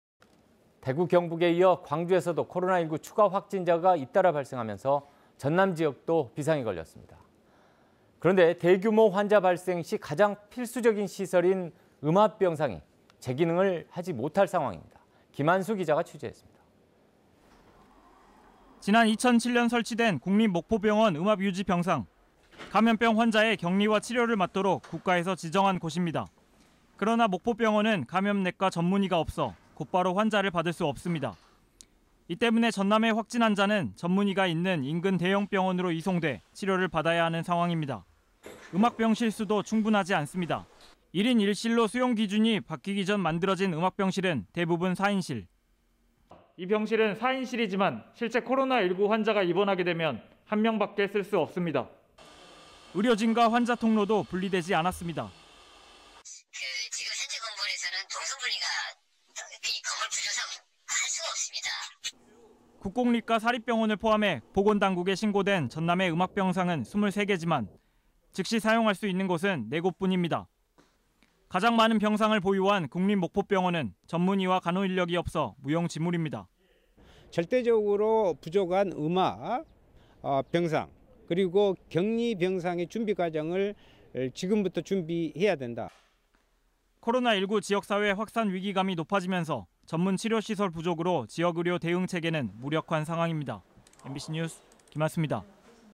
전남 음압병실 전문의 없어 '무용지물' - R(200221금/뉴스데스크)
◀ＡＮＣ▶
 대구 경북에 이어 광주에서도
코로나19 추가 확진자가 잇따라 발생하면서
전남도 비상입니다.

 대규모 환자 발생시 음압병상은
가장 필수적인 시설인데,
시설을 확인해 봤더니 제기능을 하지 못 할
상황입니다.

 김안수 기자가 현장 취재했습니다.
           ◀ＥＮＤ▶

 지난 2007년 설치된
국립목포병원 음압유지 병상.

 감염병 환자의 격리와 치료를
맡도록 국가에서 지정한 곳입니다.

 그러나 목포병원은 감염내과 전문의가 없어
곧바로 환자를 받을 수 없습니다.

 이 때문에 전남의 확진환자는
전문의가 있는 인근 대형병원으로 이송돼
치료를 받아야 하는 상황입니다.

 음압병실 수도 충분하지 않습니다.

 1인 1실로 수용기준이 바뀌기 전 만들어진
음압병실은 대부분 4인실.

S.U 이 병실은 4인실이지만
실제 코로나19 환자가 입원하게 되면
1명 밖에 쓸 수 없습니다.

 의료진과 환자 통로도 분리되지 않았습니다.

           ◀ＳＹＮ▶ 병원관계자
"지금 현재 건물에서는 동선 분리가 건물 구조상 할 수가 없습니다."

 국공립과 사립병원을 포함해 보건당국에
신고된 전남의 음압병상은 23개지만
즉시 사용 할 수 있는 곳은 4곳 뿐입니다.

 가장 많은 병상을 보유한 국립목포병원은
전문의와 간호인력이 없어 무용 지물입니다.

           ◀ＩＮＴ▶ 윤소하 국회의원
"절대적으로 부족한 음압병상 그리고 격리병상의 준비과정을 지금부터 준비해야 한다."

 코로나 19 지역사회 확산 위기감이 높아지면서 전문 치료시설 부족으로 지역의료 대응 체계는 무력한 상황입니다.
MBC뉴스 김안수입니다.